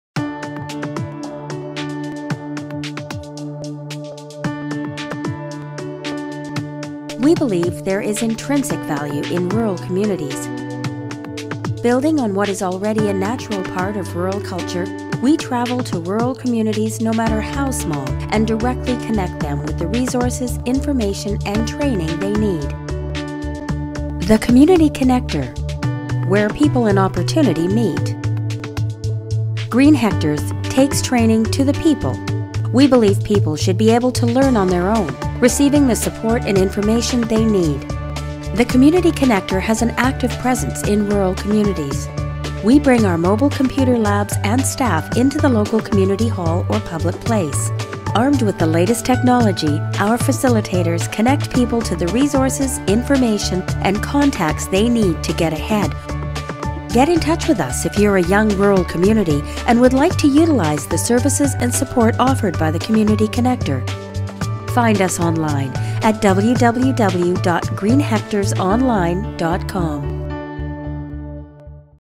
Community Connector.mov
An overview of the Green Hectares Community Connector program.